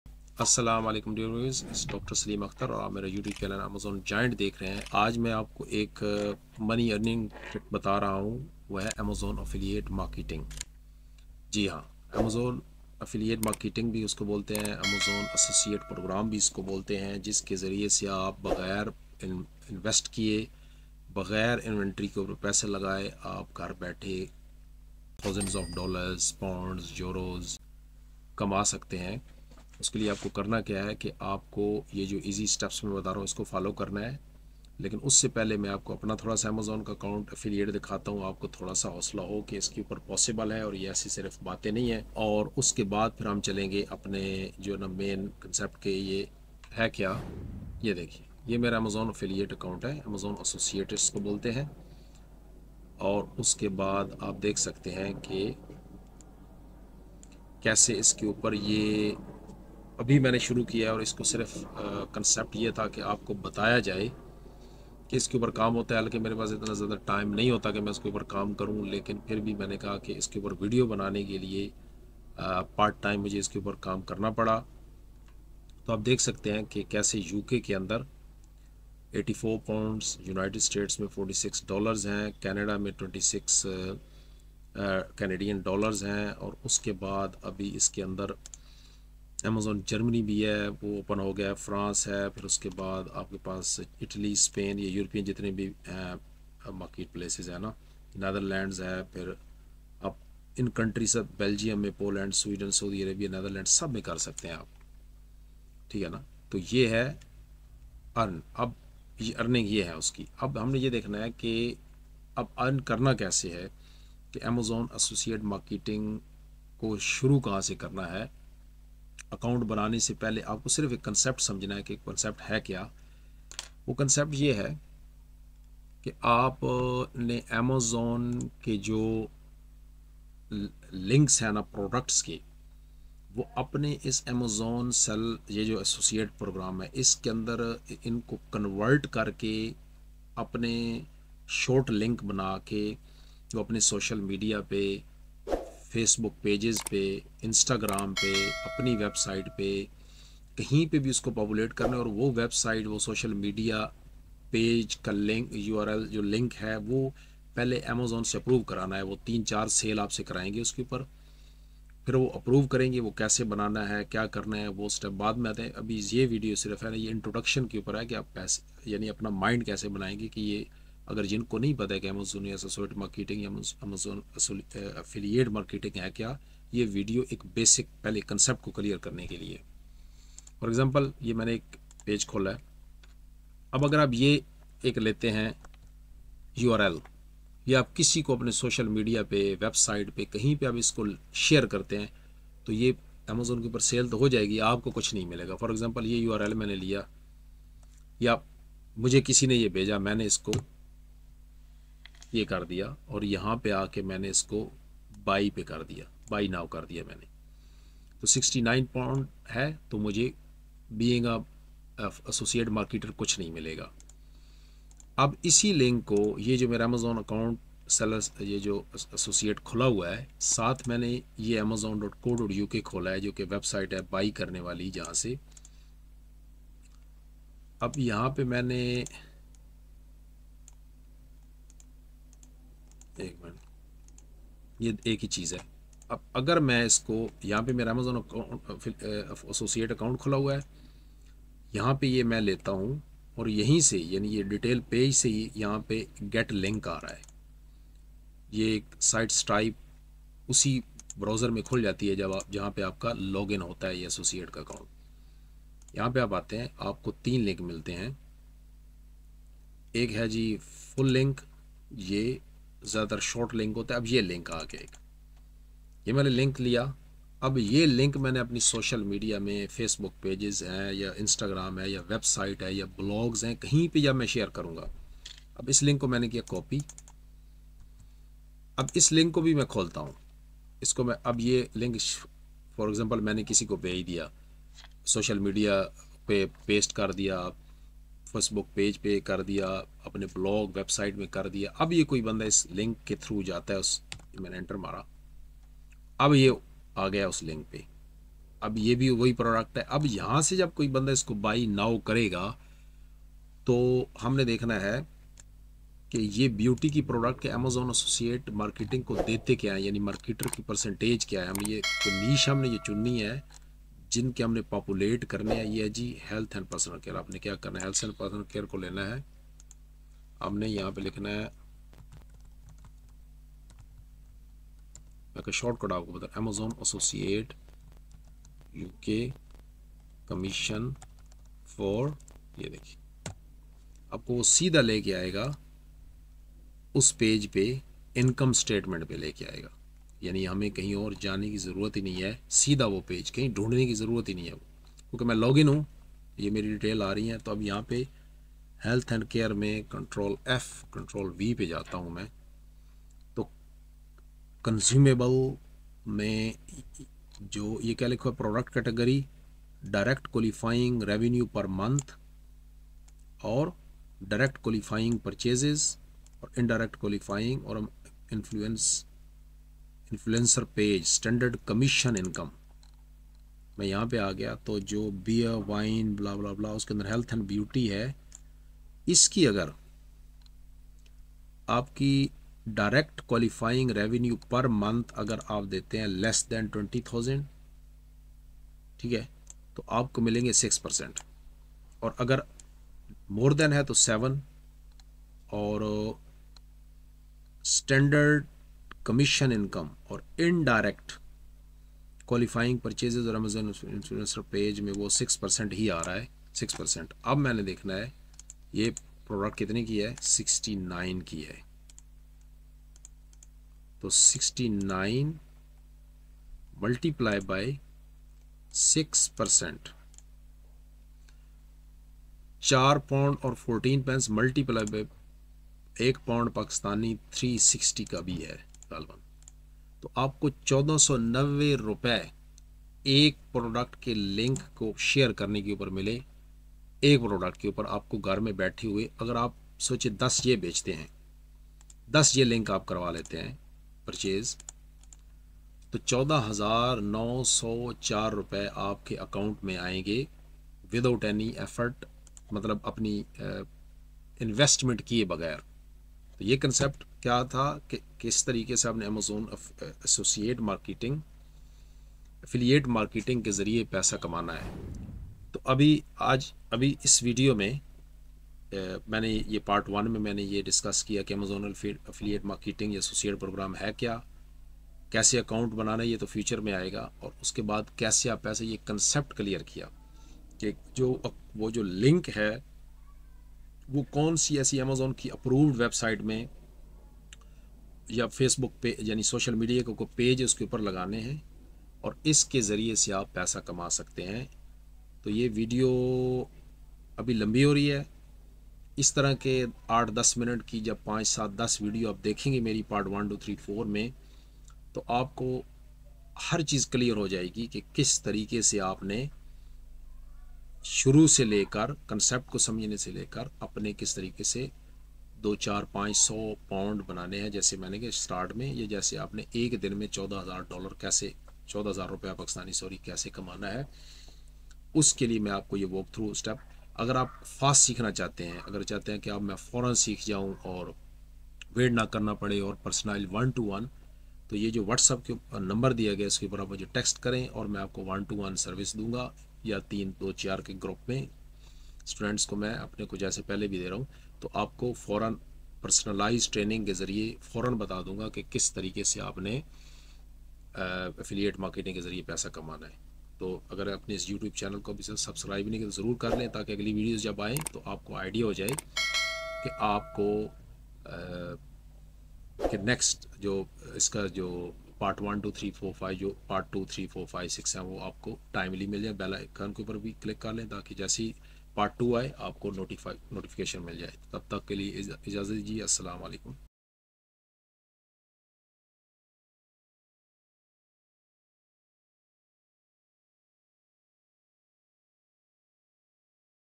Amazon Affiliate Marketing for Beginners | Free Course Part 1 | $1000/Month Strategy
Welcome to Part 1 of our comprehensive free course on Amazon Affiliate Marketing! In this series, Dr. Saleem Akhtar will guide you through the basics of affiliate marketing on Amazon, with strategies to help you earn $1000 per month. Whether you're a beginner or looking to refine your skills, this course will provide you with the knowledge and tools you need to succeed.

In this video, you'll learn:

1. Introduction to Amazon Affiliate Marketing
2. How to sign up for the Amazon Affiliate Program
3. Finding the best products to promote
4. Creating content that converts
5. Proven strategies to drive traffic and increase sales

👍 Like this video if you found it valuable.

Let's embark on this journey to make money with Amazon Affiliate Marketing together!